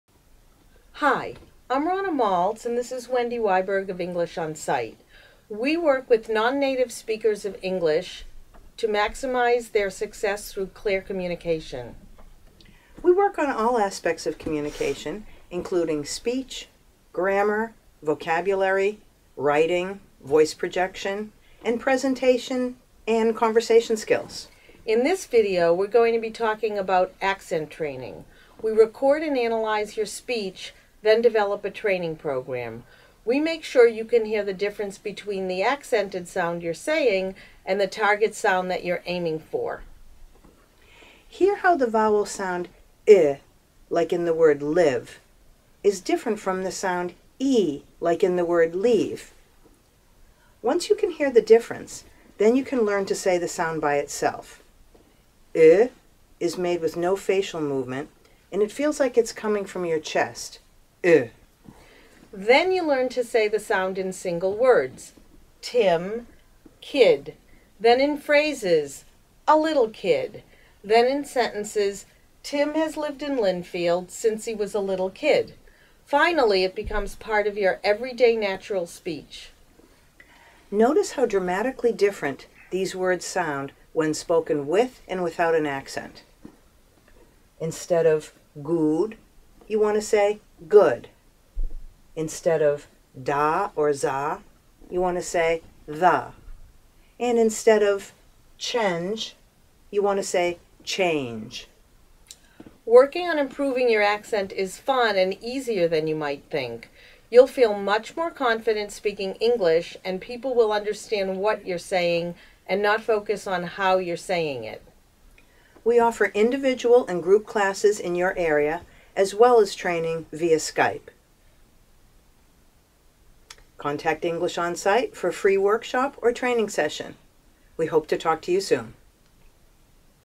English Pronunciation Training - English Onsite
This English pronunciation training by Ronna Maltz and Wendy Wiberg from English Onsite demonstrates some key strategies to improve spoken English pronunciation skills.